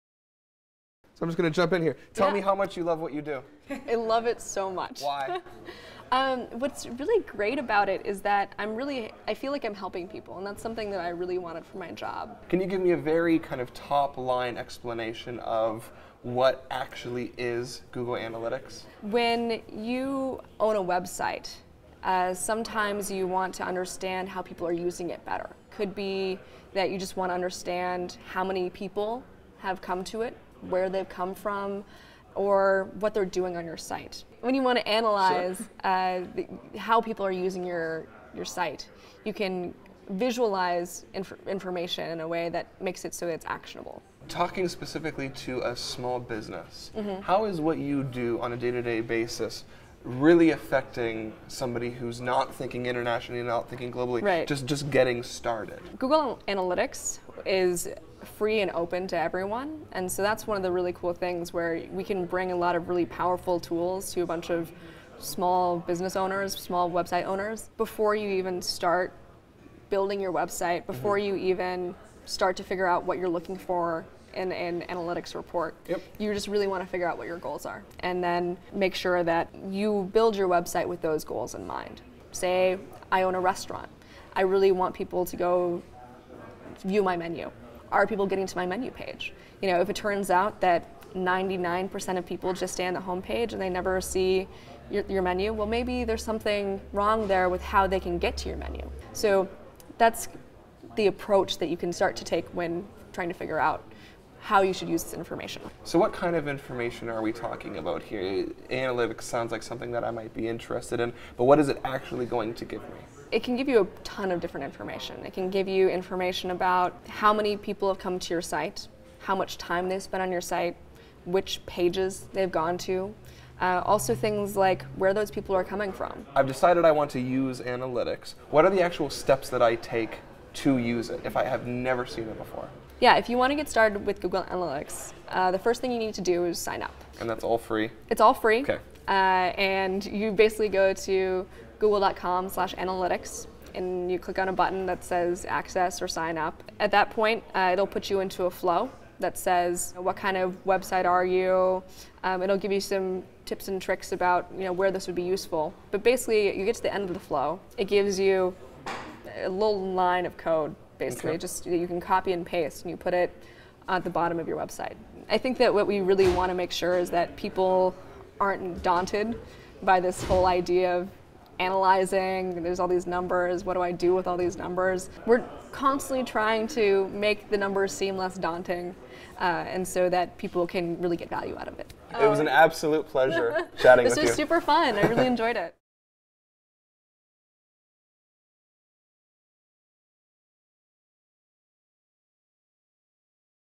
Using Google Analytics to improve your website
Laura Holmes, Product Manager on Google Analytics, explains how Google Analytics can help you better understand how users interact with your website as well as how it can help improve your website's performance.